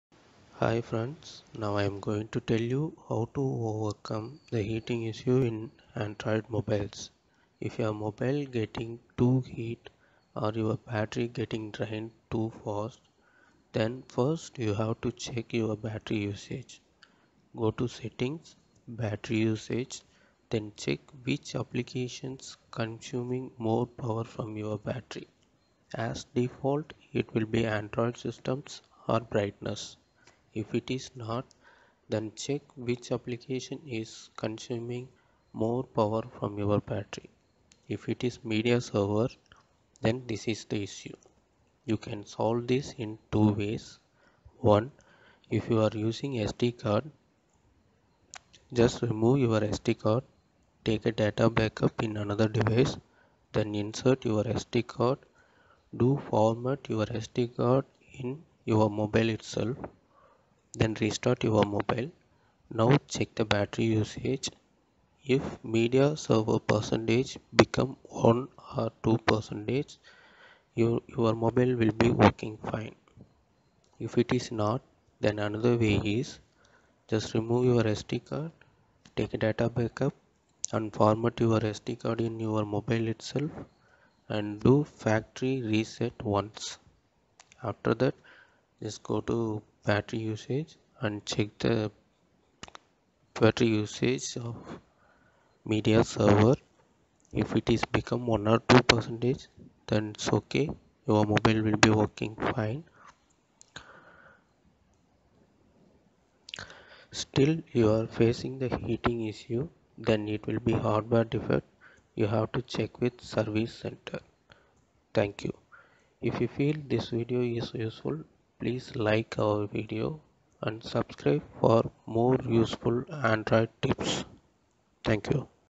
How To Solve Heating Issue In Android Mobiles
Check the procedure to solve heating issue and battery drain problem in your android mobile